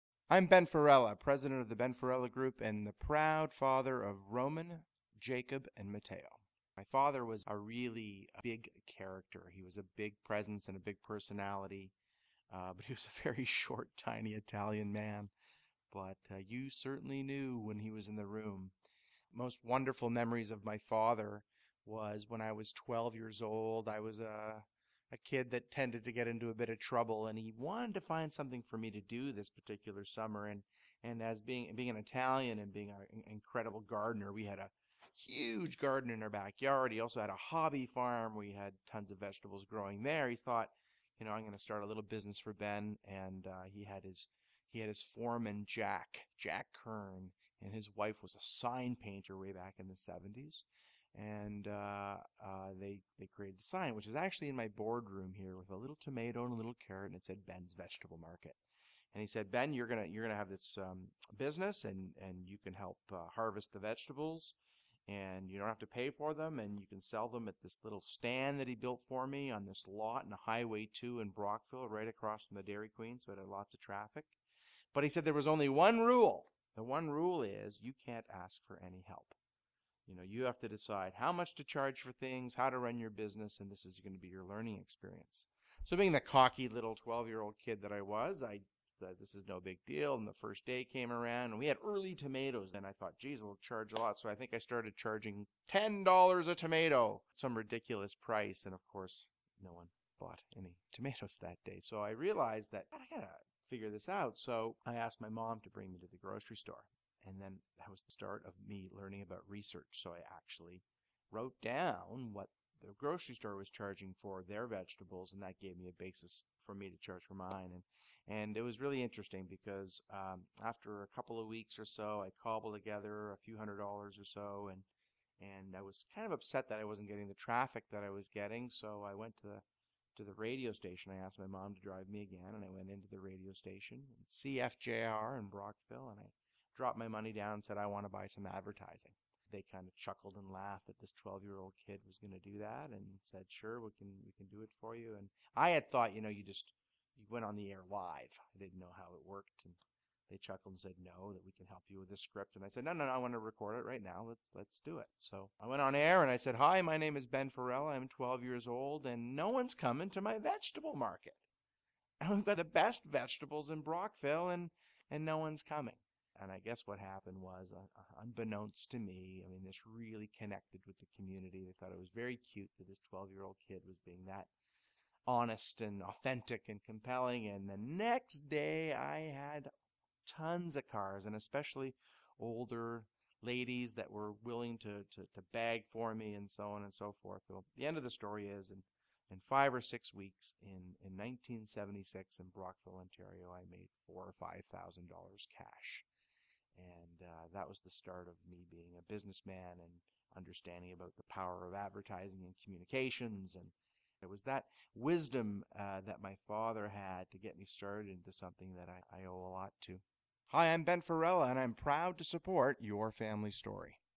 Your Family Story - Ben Farella: Vegetable Stand
Ben Farella, President of The Ben Farella Group, tells his family story about the time his father encouraged him to start up his first business selling vegetables at the age of 12. Some great lessons to be learned.